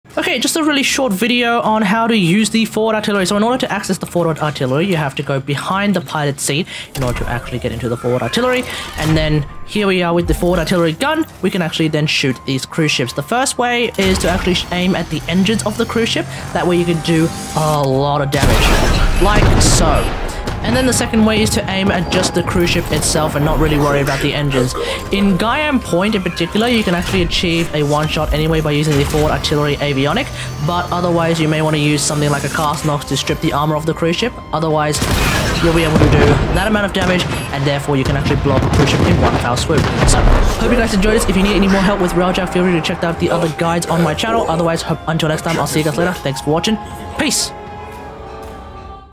How to use: The FORWARD ARTILLERY
What to think about and where to aim when considering the usage of the Forward Artillery Cannon. Once you understand this quick basic idea, you'll just need to practice.